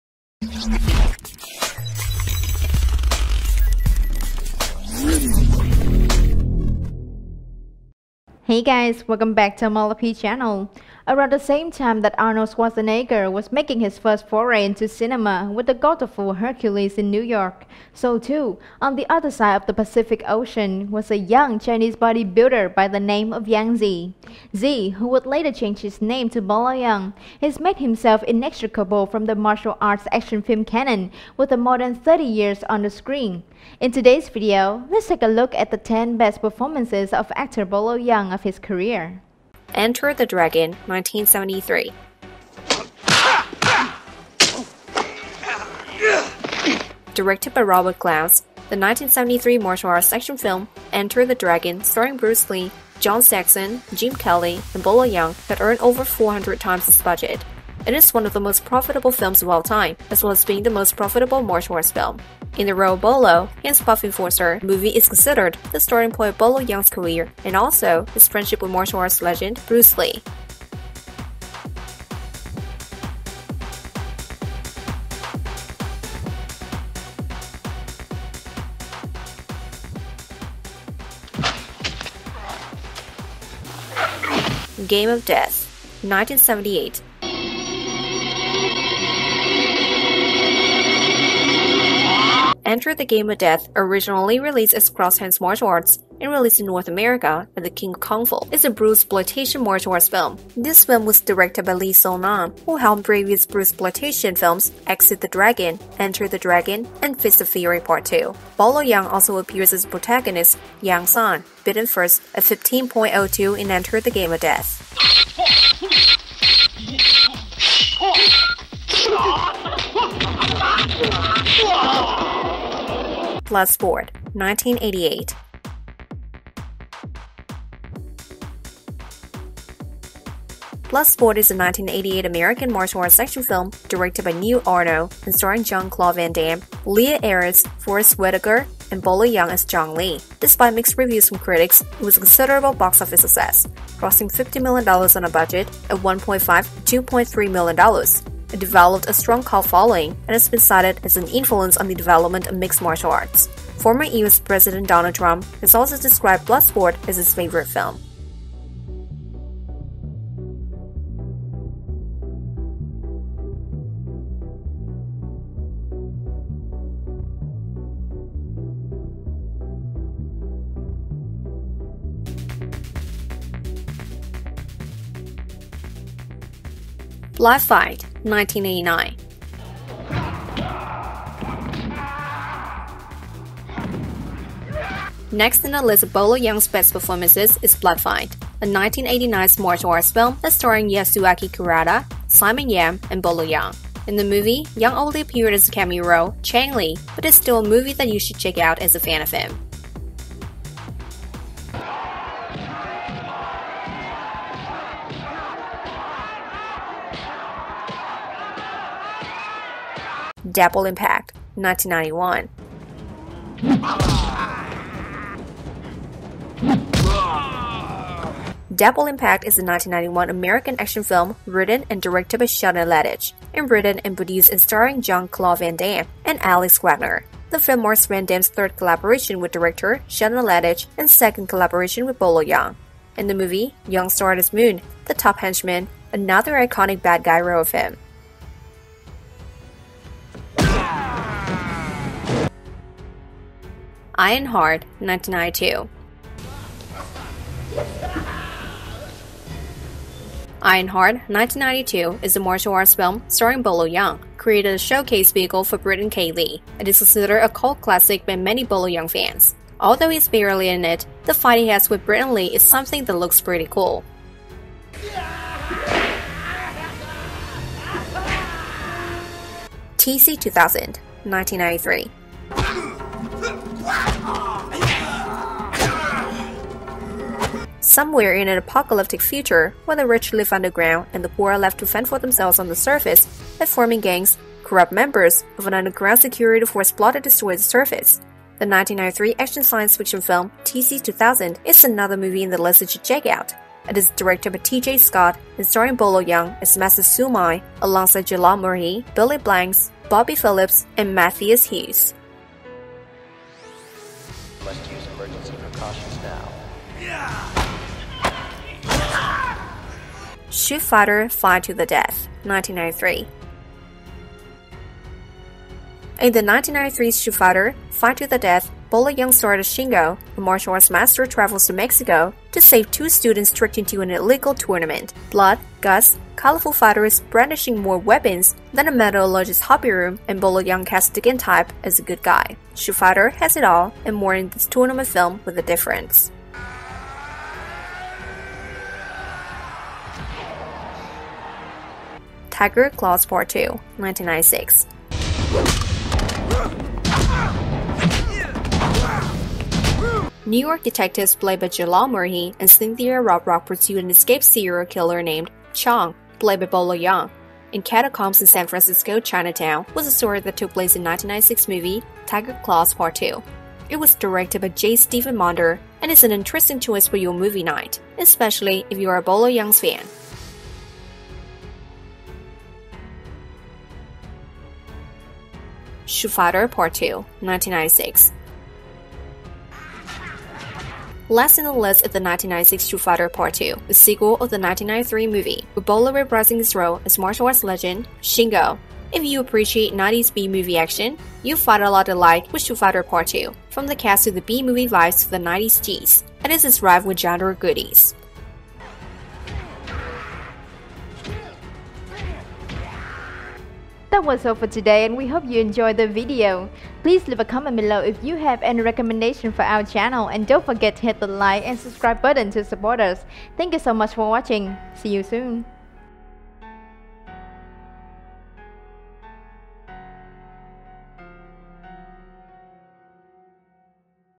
Top 10 Best Movies of Bolo Yeung
Around the same time that Arnold Schwarzenegger was making his first foray into cinema with the godawful Hercules in New York, so too, on the other side of the Pacific Ocean, was a young Chinese bodybuilder by the name of Yang Sze. Sze, who would later change his name to Bolo Yeung, has made himself inextricable from the martial arts action film canon with a more than 30 years on the screen. In today's video, let's take a look at the 10 best performances of actor Bolo Yeung in his career.